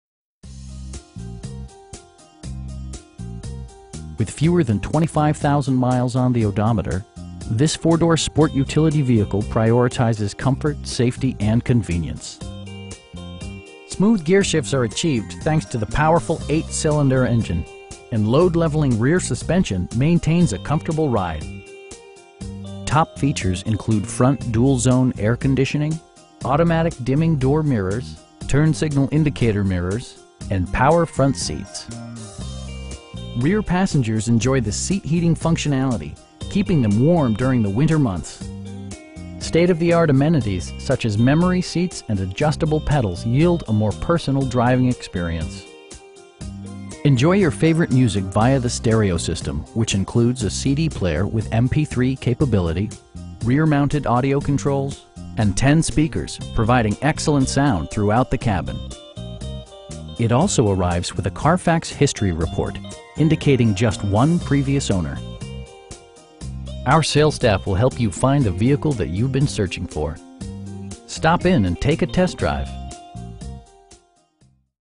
2013 CADILLAC Escalade Luxury in Houston, TX 77074
Sterling McCall Buick GMC
10422 Southwest Freeway
Building D

Take command of the road in the 2013 CADILLAC ESCALADE. With fewer than 25,000 miles on the odometer, this 4 door sport utility vehicle prioritizes comfort, safety and convenience. Smooth gearshifts are achieved thanks to the powerful 8 cylinder engine, and load leveling rear suspension maintains a comfortable ride. Well tuned suspension and stability control deliver a spirited, yet composed, ride and drive Top features include front dual zone air conditioning, leather upholstery, a tachometer, a built-in garage door transmitter, automatic dimming door mirrors, turn signal indicator mirrors, blind spot sensor, and power front seats. Power adjustable pedals allow the driver to optimize his or her driving position, enhancing visibility, comfort and safety. Rear passengers enjoy the seat heating functionality, keeping them warm during the winter months. With high intensity discharge headlights illuminating your path, you'll always appreciate maximum visibility. For drivers who enjoy the natural environment, a power moon roof allows an infusion of fresh air. State-of-the-art amenities such as memory seats and adjustable pedals yield a more personal driving experience. Enjoy your favorite music via the stereo system, which includes a CD player with MP3 capability, rear mounted audio controls, steering wheel mounted audio controls, and 10 speakers, providing excellent sound throughout the cabin. Passengers are protected by various safety and security features, including: dual front impact airbags, front SIDE impact airbags, traction control, brake assist, ignition disabling, onStar, and 4 wheel disc brakes with ABS. Safety and maximum capability are assured via self leveling rear suspension, which maintains optimal driving geometry. It also arrives with a Carfax history report, indicating just one previous owner. Our sales staff will help you find the vehicle that you've been searching for. We'd be happy to answer any questions that you may have. Stop in and take a test drive!